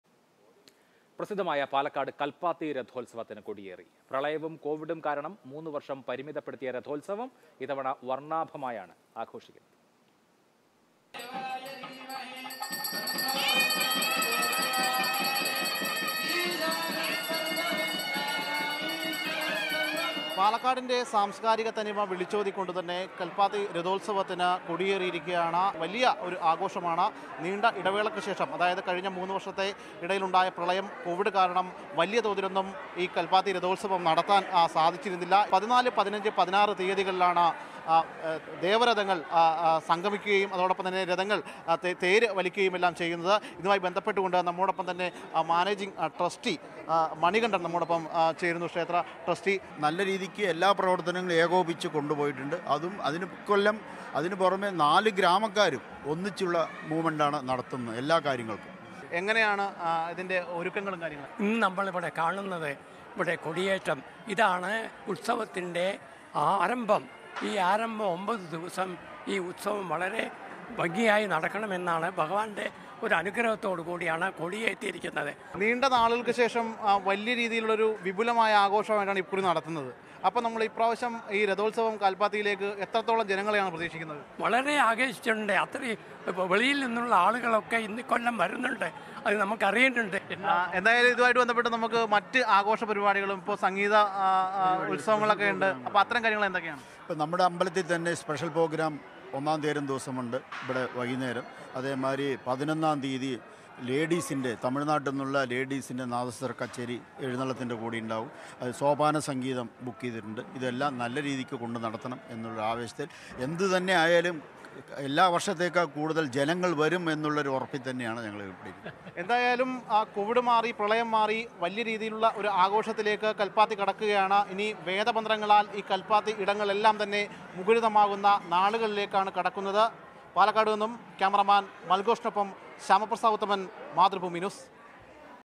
കല്‍പ്പാത്തി രഥോത്സവത്തിന് കൊടിയേറി, ആഘോഷമാക്കാനൊരുങ്ങി നാട്ടുകാര്‍
Happy viewing!